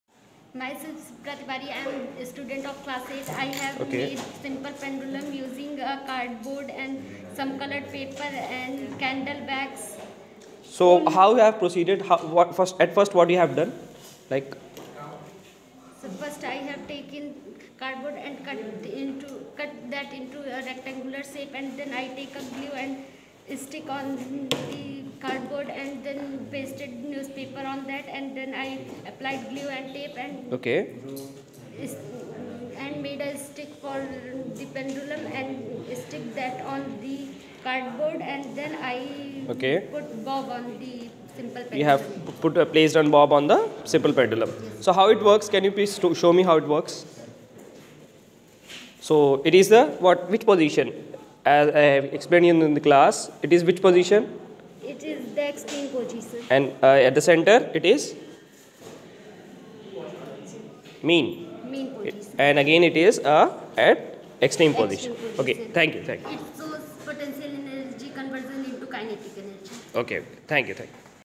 Project Working Model on Simple Pendulum...by Shipra Tiwari...Class VIII..
Demonstration @St.Johns Convent School.